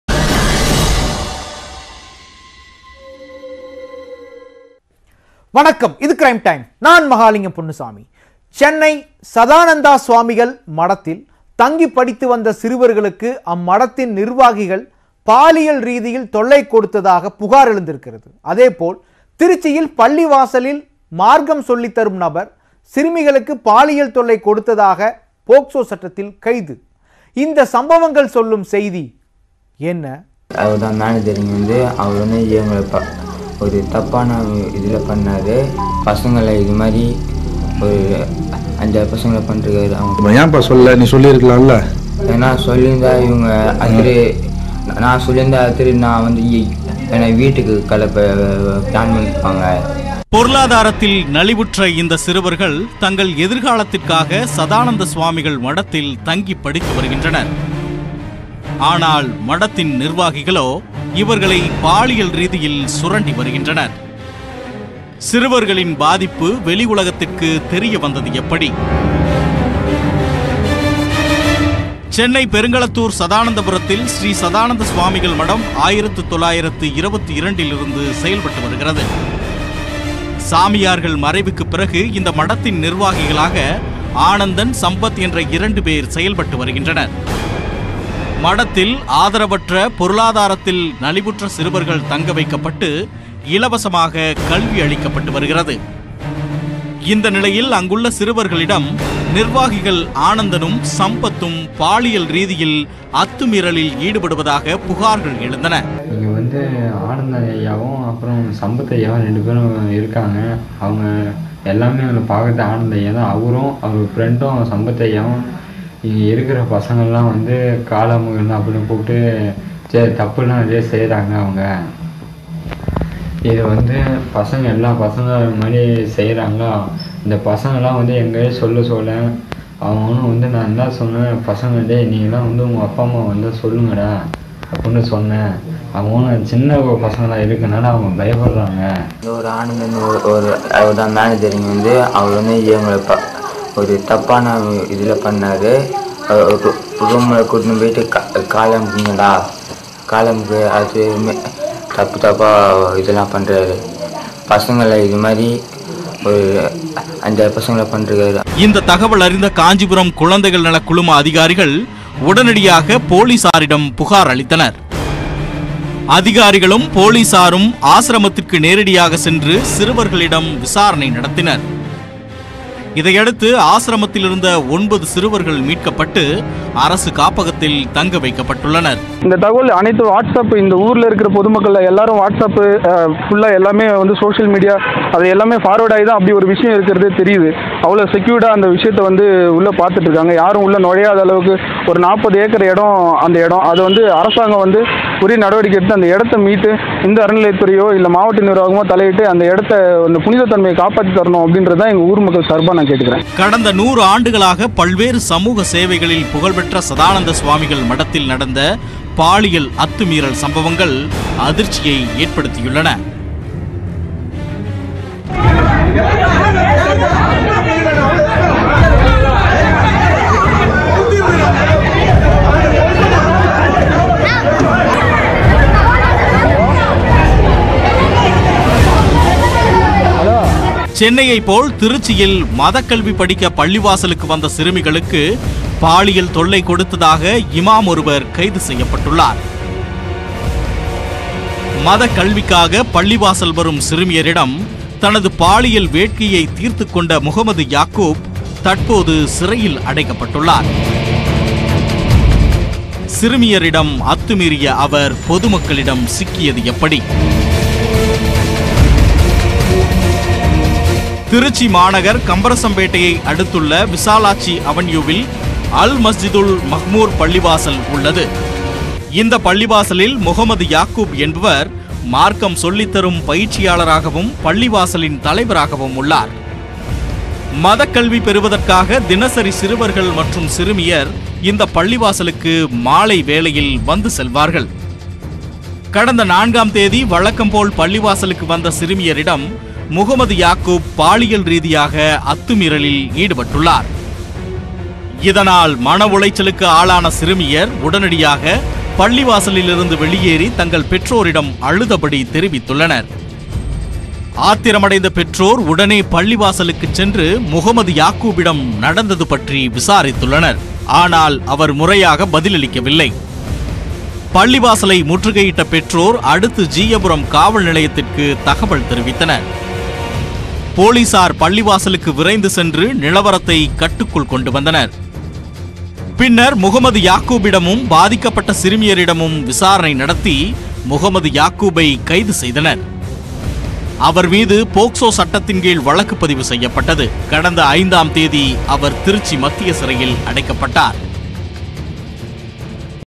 Crime Time: சென்னை ஆசிரமத்தில் சிறுவர்களுக்கு பாலியல் தொல்லை - வெளியான பகீர் வாக்குமூலம்
சென்னை புறநகரில், மடத்தில் தங்கிய சிறுவர்களுக்கு ஓரினச்சேர்க்கை தொல்லை எனப் புகார் எழுந்துள்ளது.